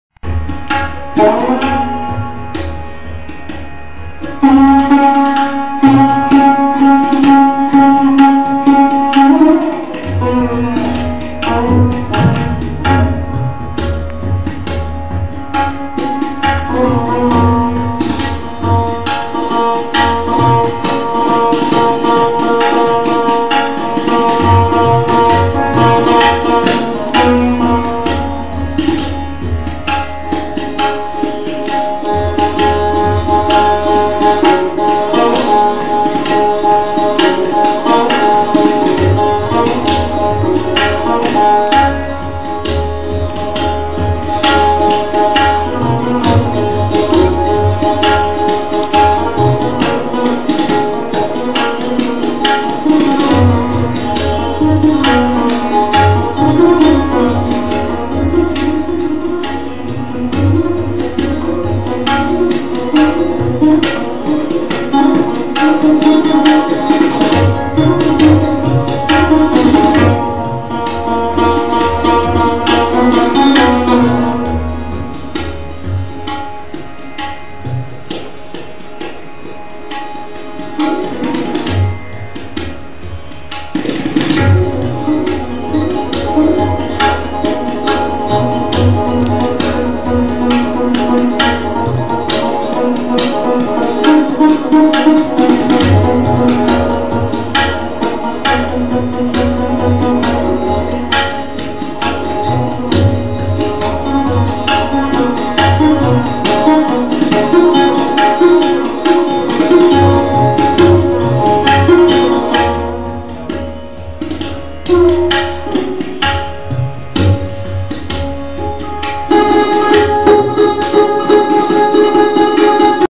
Pt Partho Sarothy Chowdhury With Pt Ananda Gopal Bandopadhyay House Concert 2007 Jhinjhorty Part2
Pandit Partho Sarothy Chowdhury With Pandit Anandfa Gopal Bandopadhyay  Live At House Concert Kolkata On 29th Sep 2007 Raag JhinJhoti...Part 2